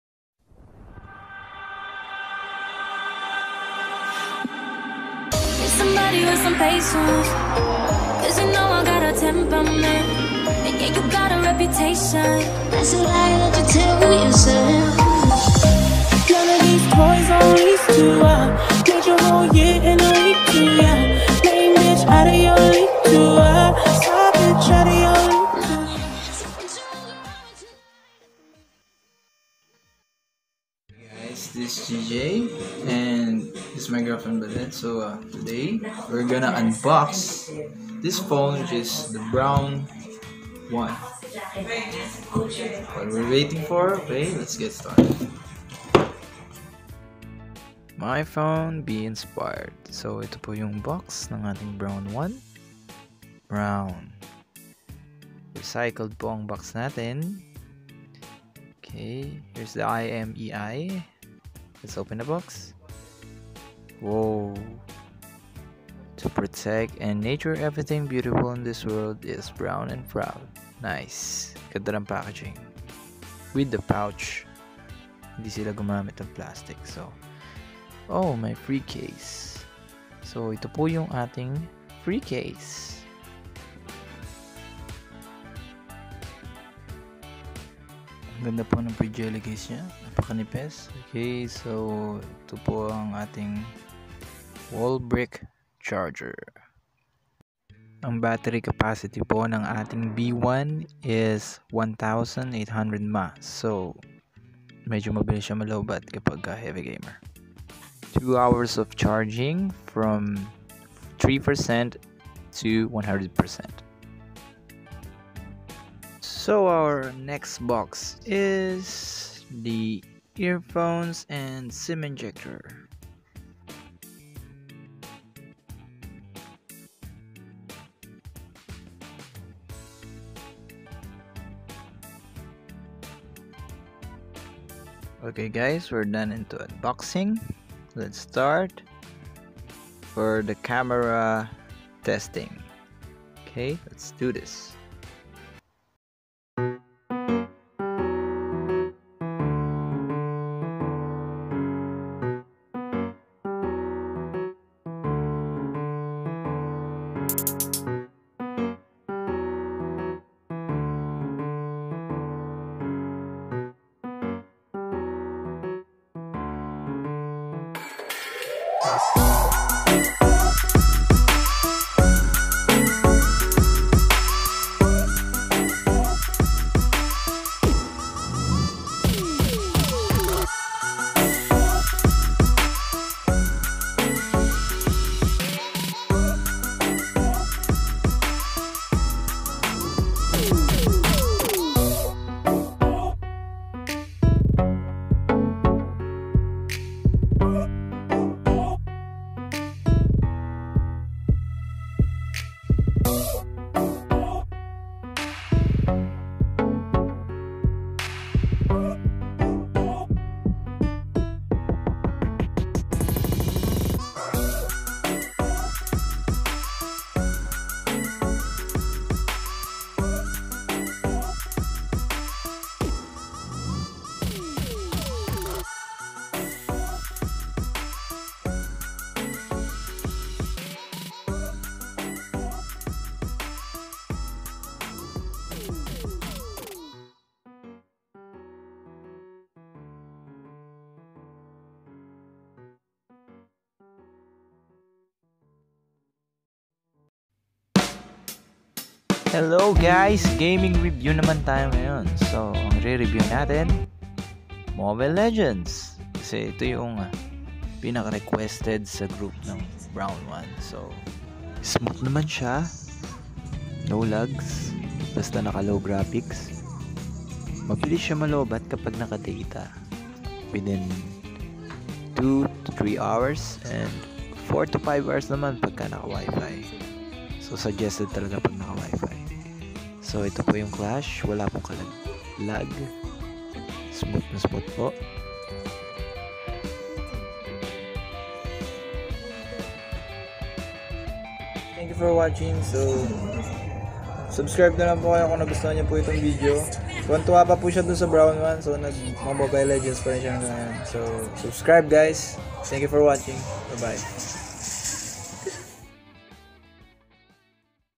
Myphone Brown 1 /unboxing, and review.
P2,999 best budget phone

Specs

4.7" HD IPS Display
1.3GHz Quad-Core
16GB Internal Storage expandable up to 64GB via microSD card slot
2GB RAM
13MP Main Camera
8MP Front Camera
4G LTE
1800mAh Battery
Fingerprint Scanner
USB OTG
Android™ 7.0 Nougat